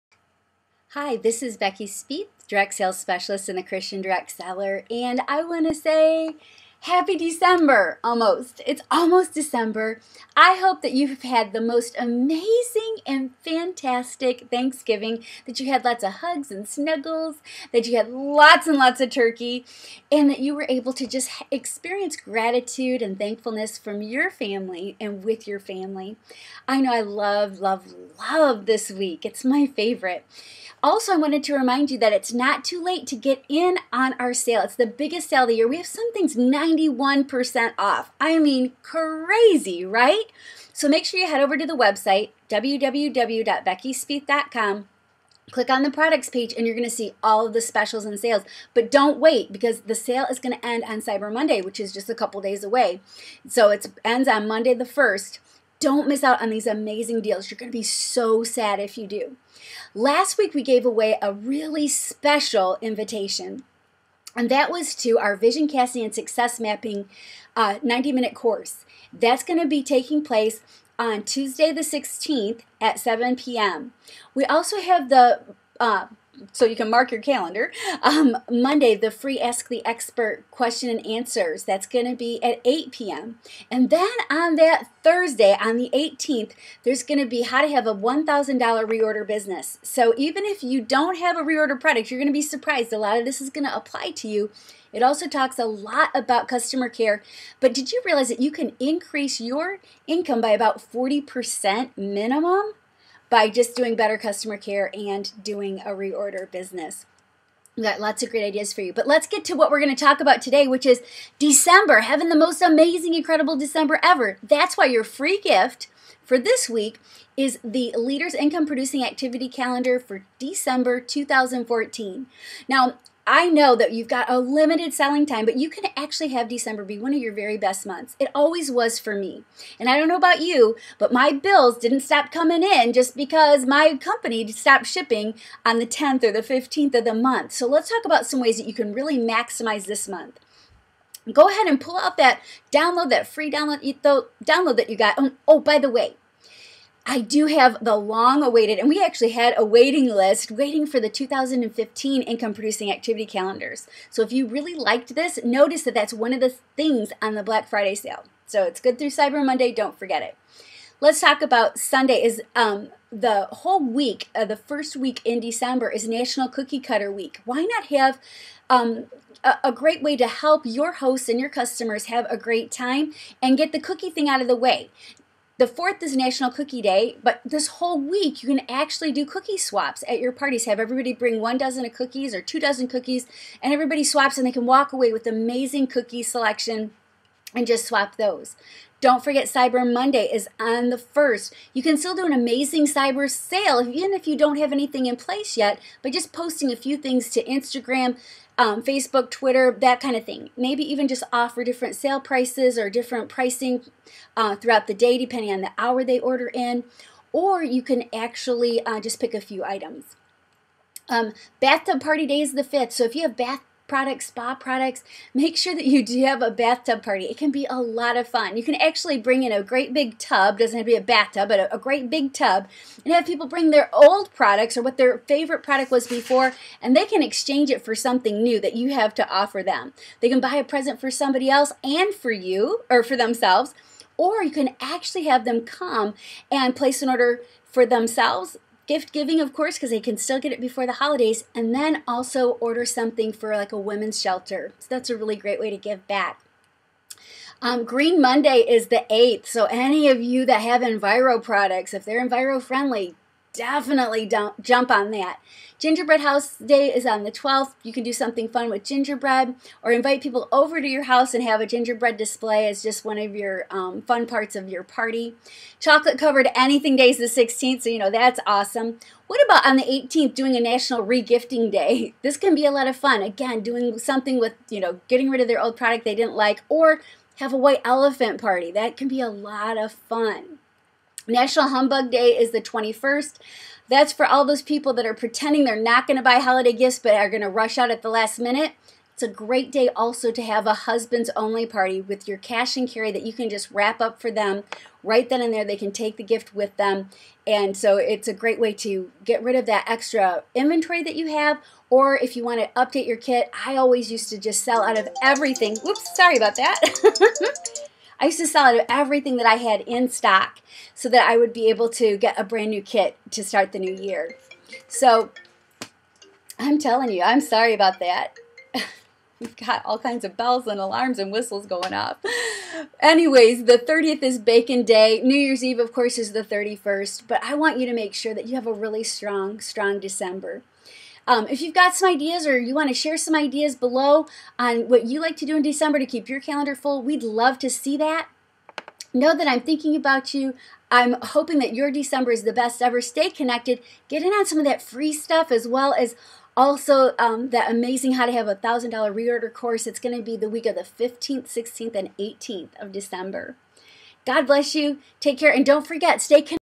How to Have the Best December Yet
Becky Spieth, Direct Sales Specialist also known as "The Christian Direct Seller" shares how to maximize your December direct selling business.  There are also several other FREEBIES offered in this video to support your business for December as well as how to have the BEST New Year in business!!  Share below what your favorite idea is to have a strong December in business.